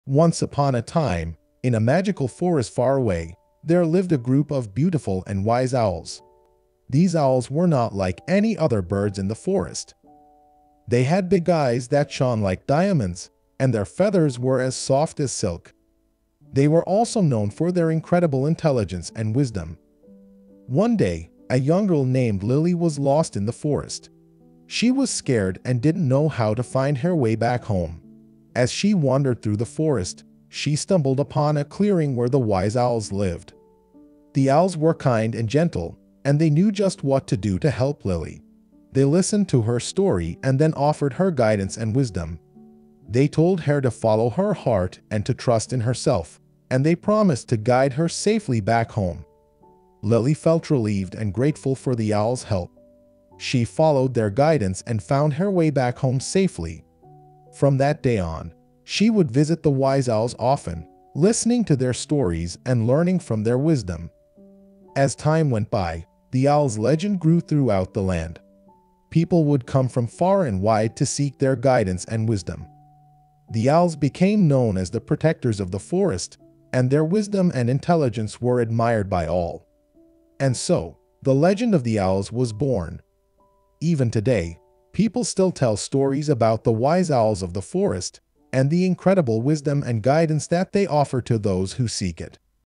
The legend of the nameless owl
Explore the enchanting world of wise and mysterious owls, known for their big eyes, soft feathers, and special ability to see through the darkness of the night. Learn about the legend of owls and their unique hunting skills, as well as their symbolism in ancient cultures as protectors and symbols of good luck. The story of the owls is a timeless tale that continues to inspire us with their intelligence and beauty.